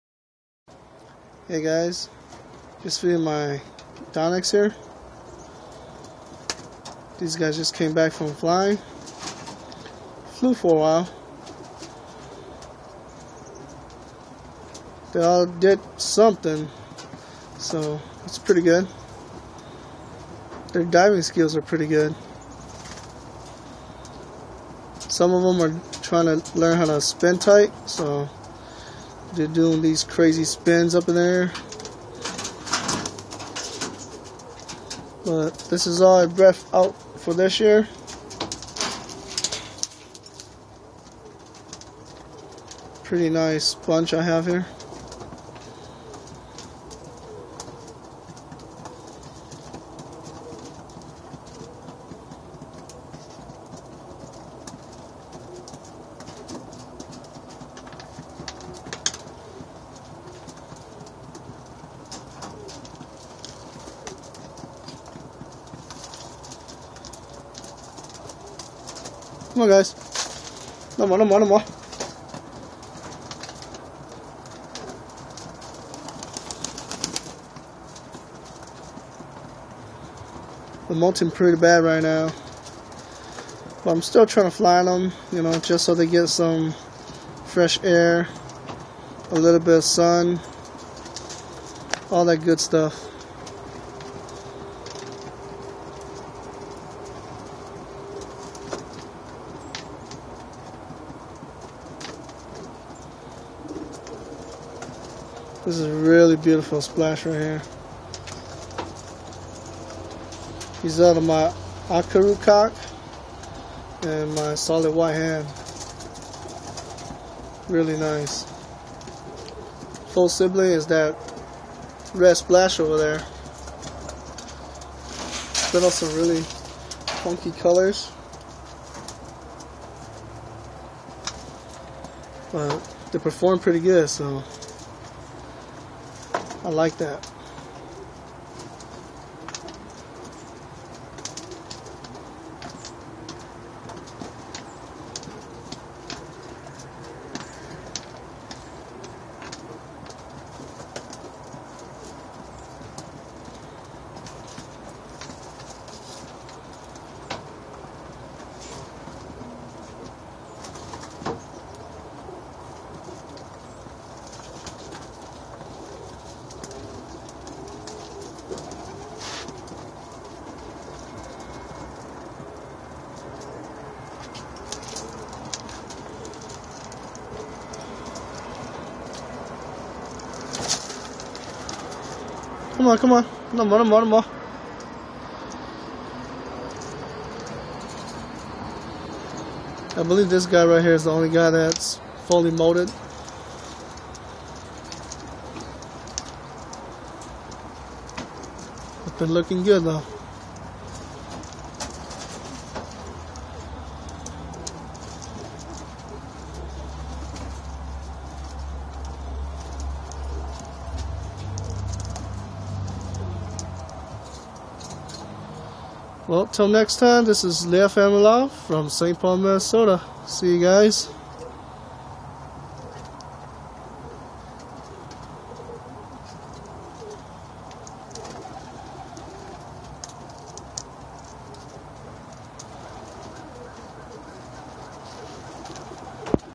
Liab Family Loft - Feeding My Donek Team
These guys did excellent today! They were spinning and diving all over the place. They flew for a while before they dived down. It was amazing! These guys get to eat to the fullest and they still put on a show. Can't wait to see them improve more! Thanks!